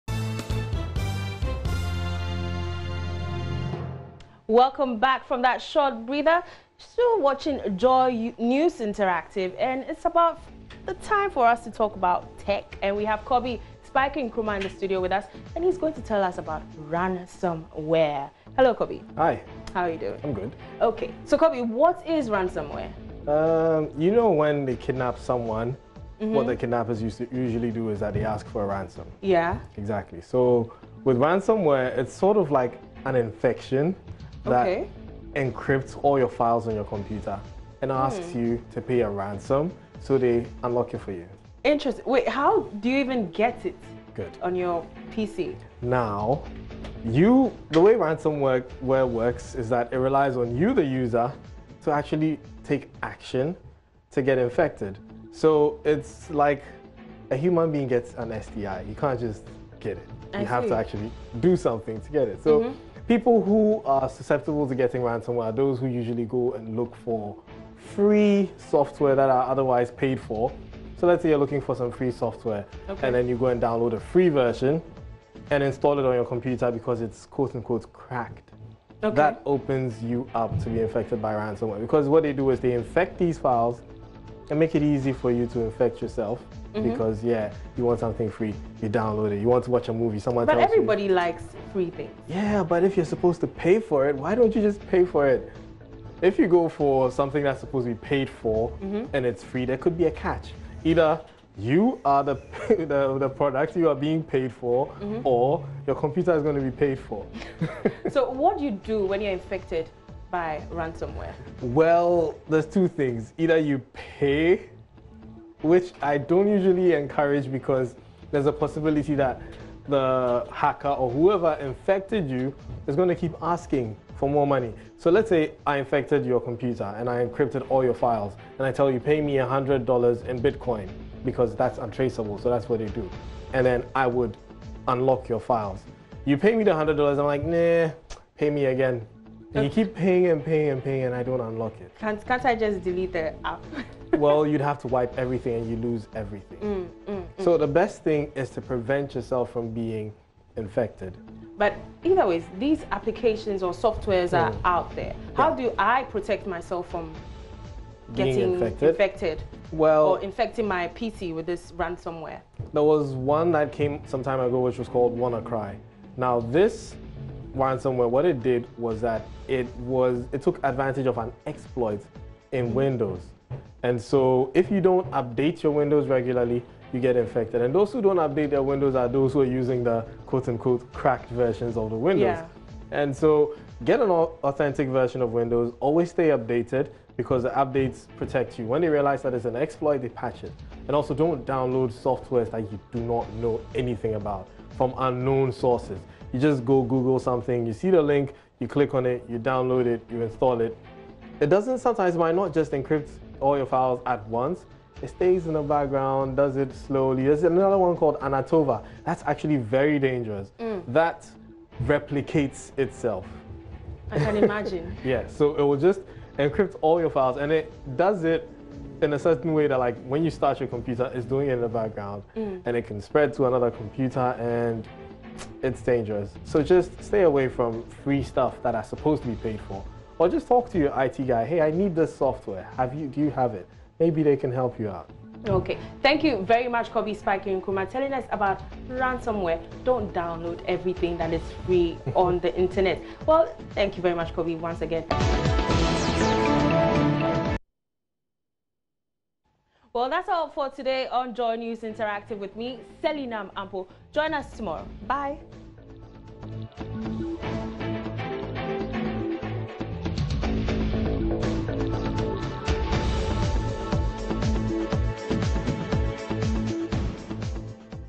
Tech Talk: Ransomware - JoyNews Interactive (27-2-19)
Ransomware: How to protect your device against it